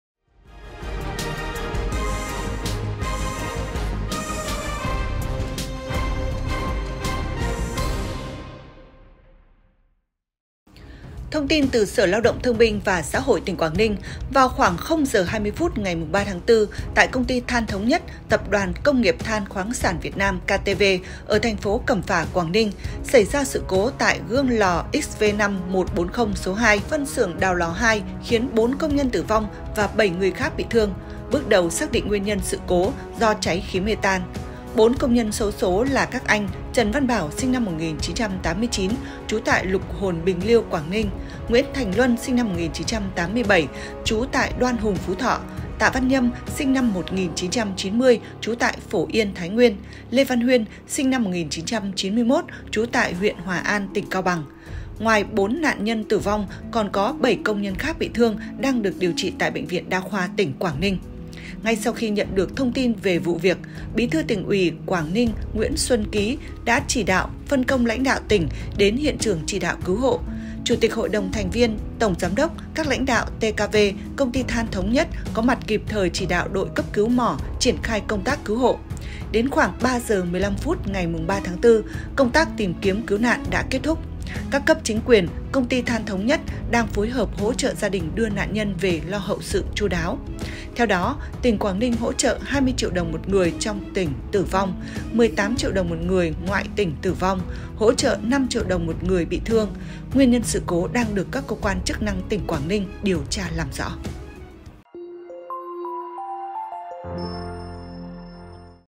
Quảng Ninh: Xót xa 4 công nhân tử vong do sự cố hầm mỏ | CafeLand
CafeLand | Thông tin từ Sở Lao động - Thương binh và Xã hội tỉnh Quảng Ninh, vào khoảng 0 giờ 20 phút ngày 3/4 tại Công ty Than Thống Nhất (Tập đoàn Công nghiệp Than - Khoáng sản Việt Nam - KTV) ở thành phố Cẩm Phả (Quảng Ninh) xảy ra sự cố tại gương lò XV5-140 số 2, Phân xưởng Đào lò 2 khiến 4 công nhân tử vong và 7 người khác bị thương. Bước đầu xác định nguyên nhân sự cố do cháy khí metan.
Bốn công nhân xấu số là các anh Trần Văn Bảo (sinh năm 1989, trú tại Lục Hồn, Bình Liêu, Quảng Ninh), Nguyễn Thành Luân ( sinh năm 1987, trú tại Đoan Hùng, Phú Thọ), Tạ Văn Nhâm (sinh năm 1990, trú tại Phổ Yên, Thái Nguyên), Lê Văn Huyên (sinh năm 1991, trú tại huyện Hòa An, tỉnh Cao Bằng). Ngoài 4 nạn nhân tử vong còn có 7 công nhân khác bị thương đang được điều trị tại Bệnh viện Đa khoa tỉnh Quảng Ninh. Ngay sau khi nhận được thông tin về vụ việc, Bí thư Tỉnh ủy Quảng Ninh Nguyễn Xuân Ký đã chỉ đạo, phân công lãnh đạo tỉnh đến hiện trường chỉ đạo cứu hộ. Nguyên nhân sự cố đang được các cơ quan chức năng tỉnh Quảng Ninh điều tra làm rõ.